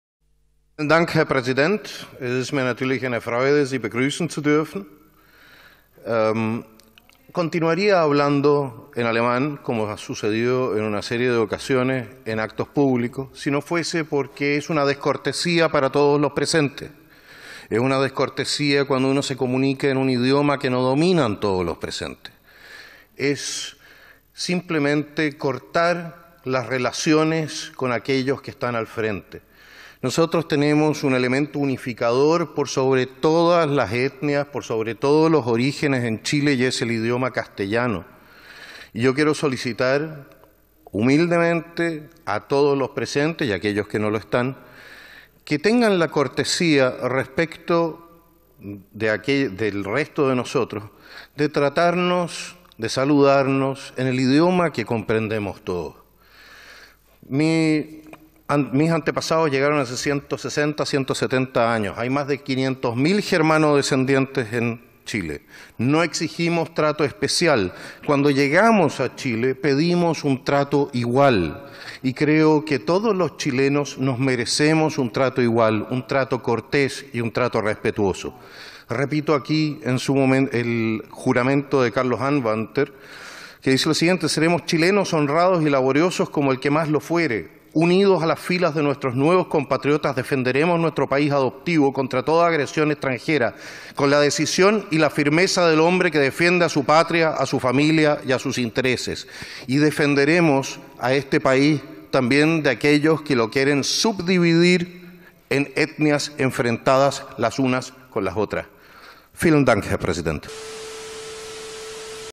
HABLANDO ALEMÁN: el polemico discurso del diputado Johannes Kaiser para pedir el uso del castellano
El diputado JohannesKaiser intervino en la Cámara de Diputadas y Diputados en alemán para hablar de descortesía y pedir que se hable "en el idioma que comprendemos todos".

Durante su intervención por la bancada del Partido Republicano, fundado por el ex candidato presidencial José Antonio Kast, Kaiser aseguró que podría seguir hablando en alemán "si no fuese porque es una descortesía para todos los presentes".

"Es una descortesía cuando uno se comunica en un idioma que no dominan todos los presentes, es simplemente cortar las relaciones con aquellos que están al frente", recalcó el diputado.